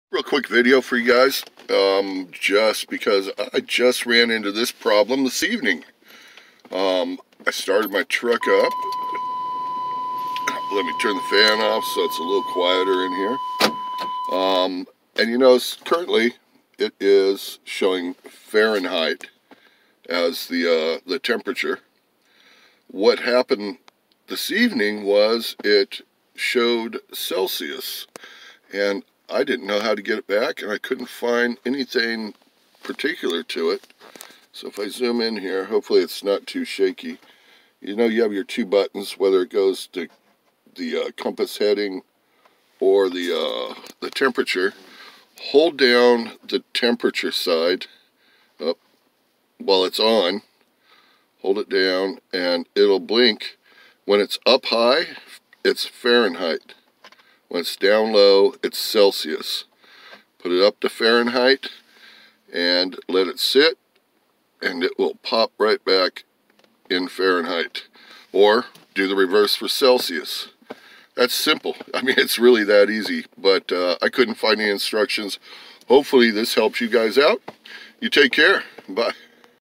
1992 to 1999 Chevy GMC mirror Celcius to Fahrenheit Temperature
This is how to change the temperature display on your rearview mirror. this is my 1998 GMC Suburban but I believe it is the same for all GMC and Chevy Trucks and SUVs from 1992 thru 1998.